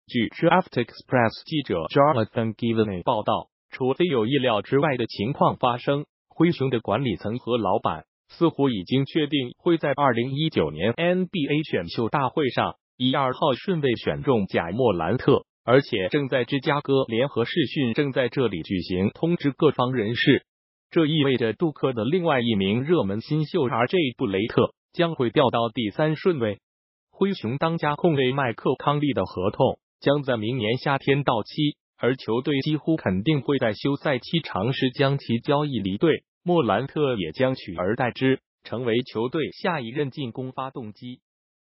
灰熊的榜眼签已经基本锁定莫兰特
据DraftExpress记者Jonathan Givony报道，除非有意料之外的情况发生，灰熊的管理层和老板似乎已经确定会在2019年NBA选秀大会上以2号顺位选中贾-莫兰特，而且正在芝加哥（联合试训正在这里举行）通知各方人士。这意味着杜克的另外一名热门新秀RJ-布雷特将会掉到第三顺位。